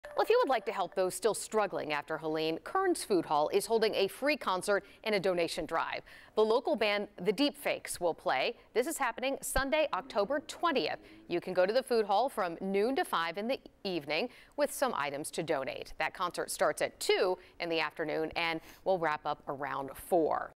Free concert, donation drive for Helene flood relief at Kern's Food Hall
Local band The Deep Fakes is partnering with Mercedes-Benz of Knoxville and Monday Night Brewing to hold the event.